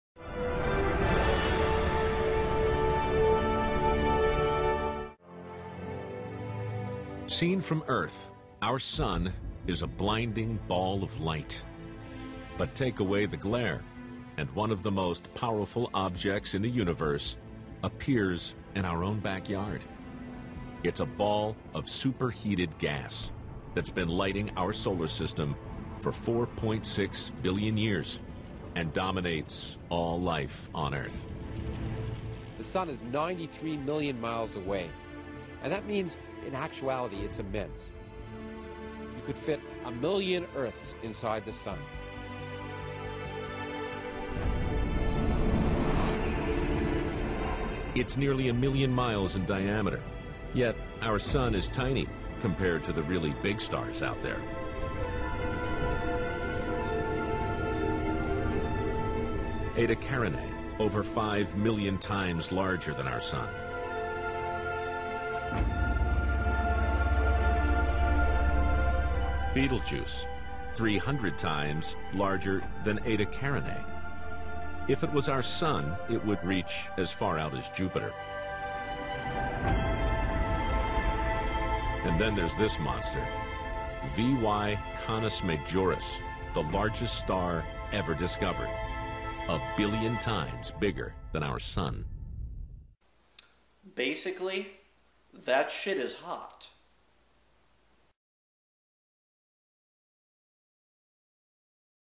The BIGGEST Star
Is our sun the biggest star? Ask Nick Spears...